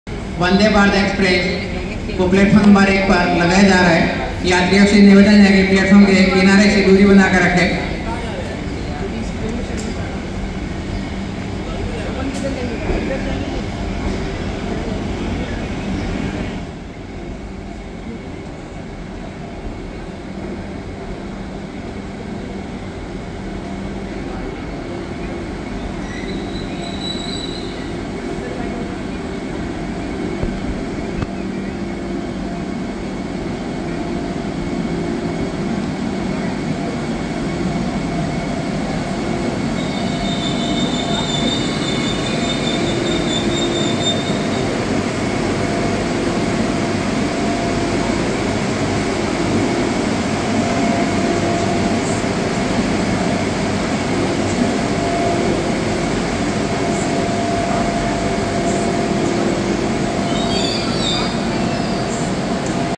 Jodhpur Sabarmati Vande Bharat Express coming into Platform for Journey...!!
12461 Jodhpur Sabarmati Vande Bharat Express shunting into PF No. 1 for Journey to Ahmedabad.

जोधपुर साबरमती वंदे भारत एक्सप्रेस।